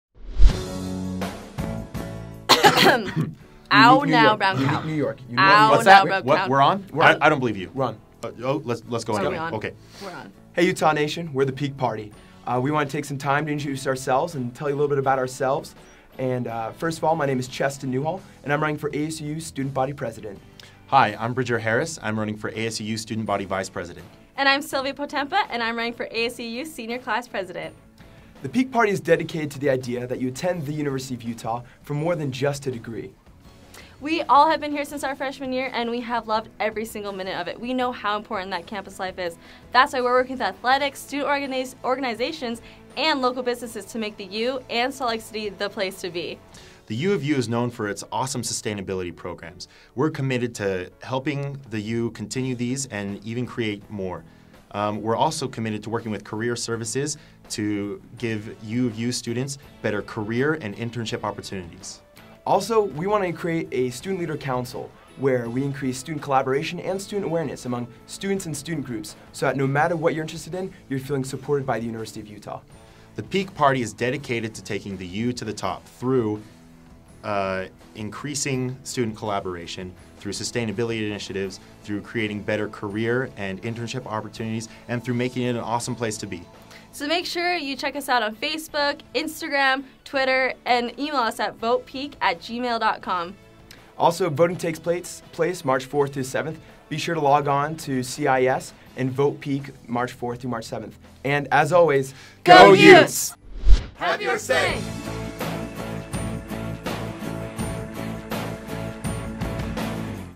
Have Your Say: The Peak Party
University of Utah students - ASUU Student Government elections will occur March 4-7. Have your say in selecting a student leader to represent you and student issues across campus. The PEAK PARTY is dedicated to the idea that students come to the University of Utah for more than just a degree. Check out the Peak Party candidates and find out how to vote: asuu.utah.edu/elections2013.